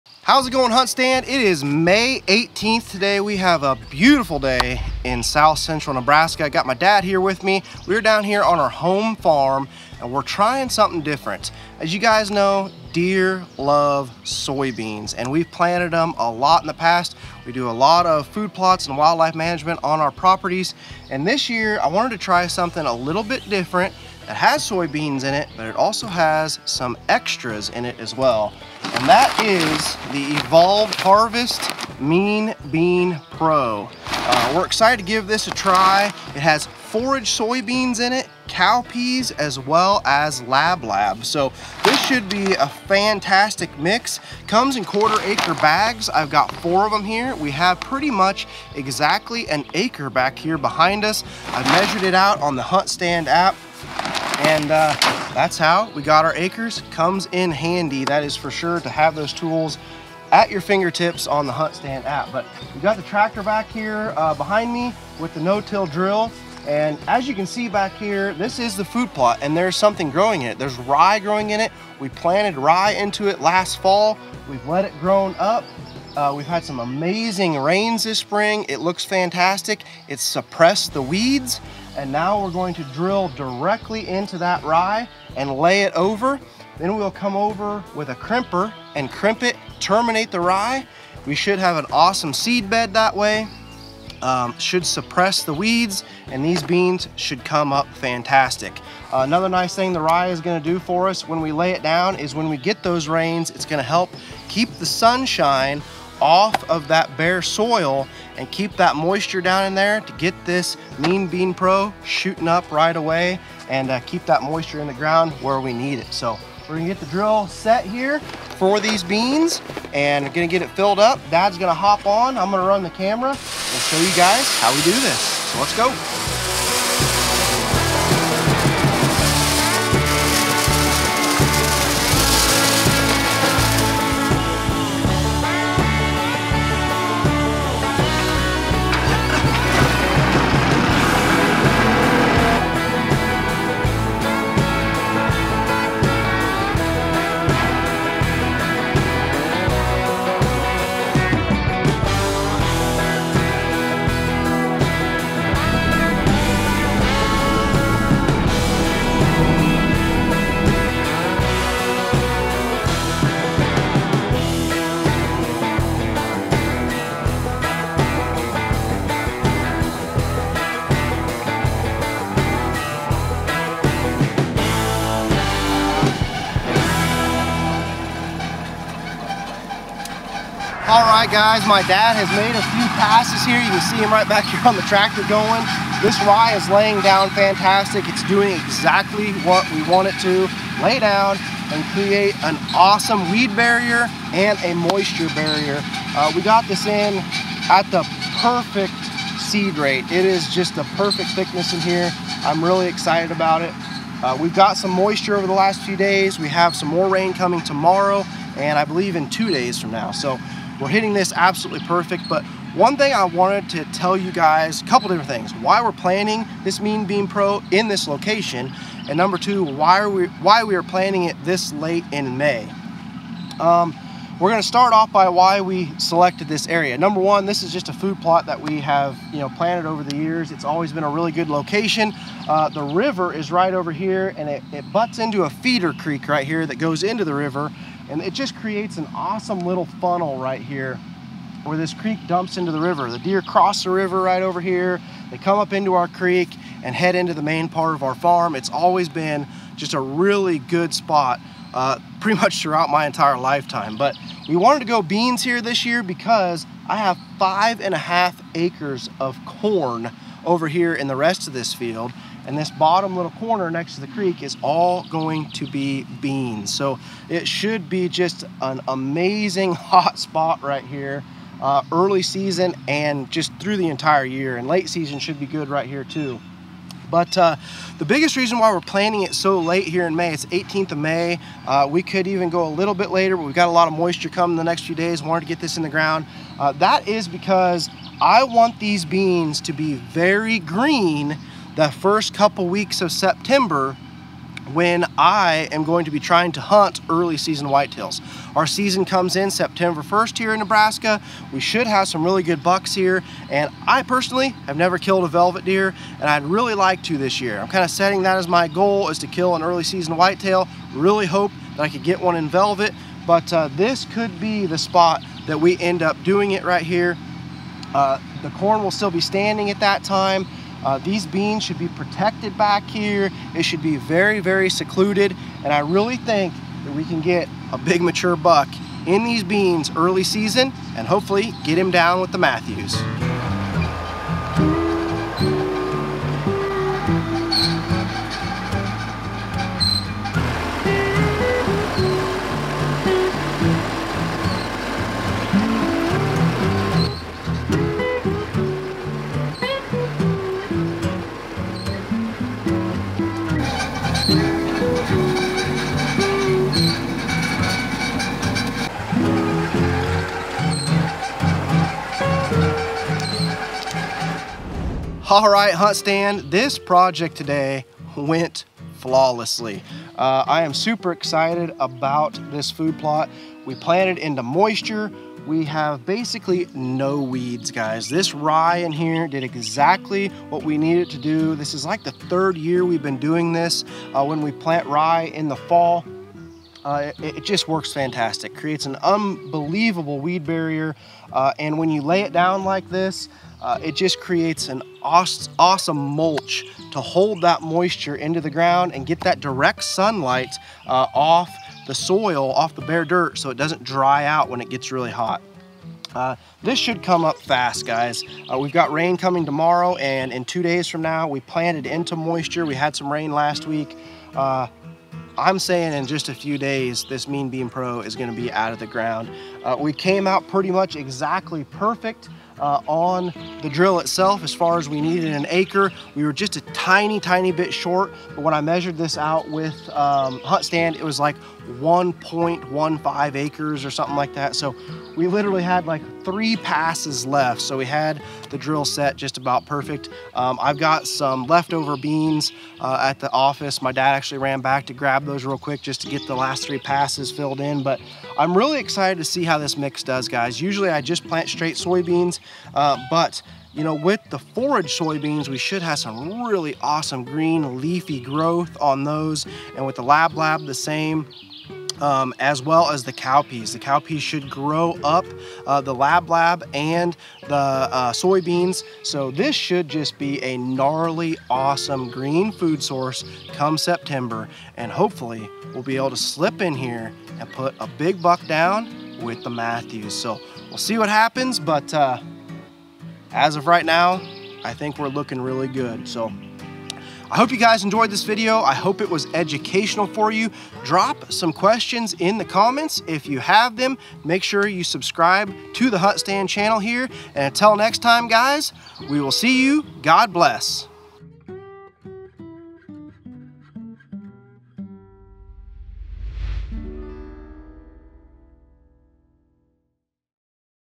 How To Create the Ultimate Early Season Kill Plot with Evolved Mean Bean Pro
Join HuntStand Pro Contributor Cody Butler as he shares tips for creating the ultimate early season kill plot using a no-till method with standing rye. He explains how this approach suppresses weeds, retains moisture, and builds soil, setting the stage for a lush green food plot by early September for the opening of deer season in Nebraska.